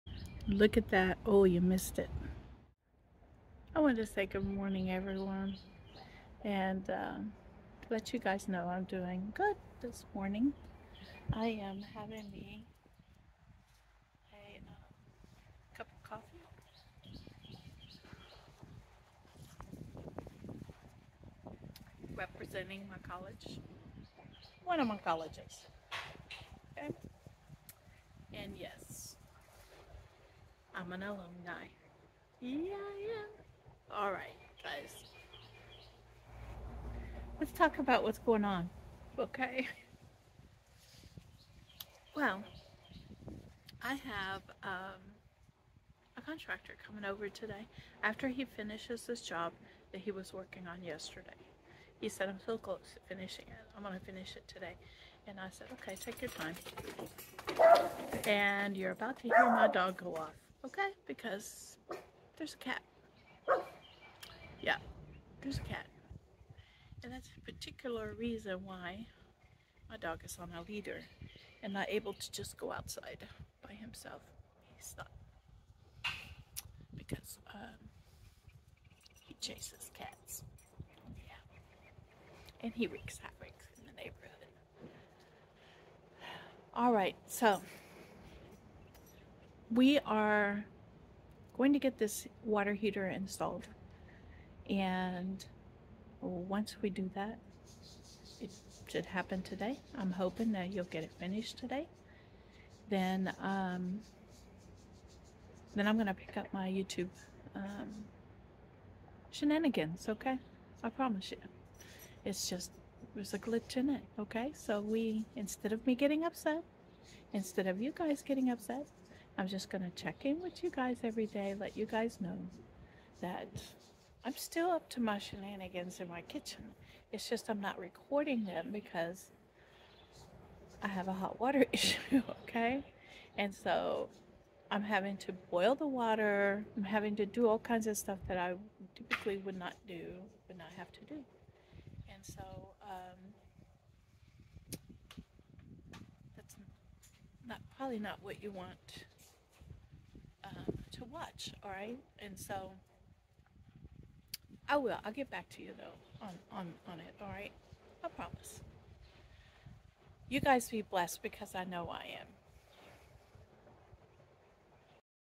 Caught a black cat in a Halloween Decoration this morning. Good Morning
Just wanted to touch base with you guys and gals and let you know what is happening today.